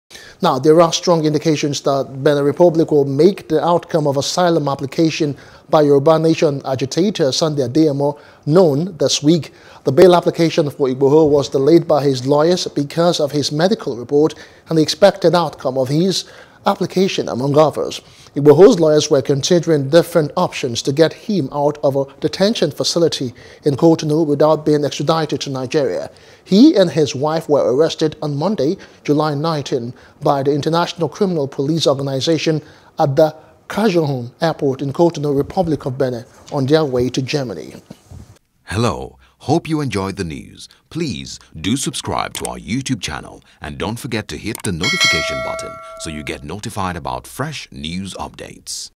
Benin Republic To Determine Sunday Igboho’s Request This Week | NEWS
There are strong indications that Benin Republic will make the outcome of asylum application by Yoruba Nation agitator, Sunday Adeyemo, popularly known as Sunday Igboho, known this week.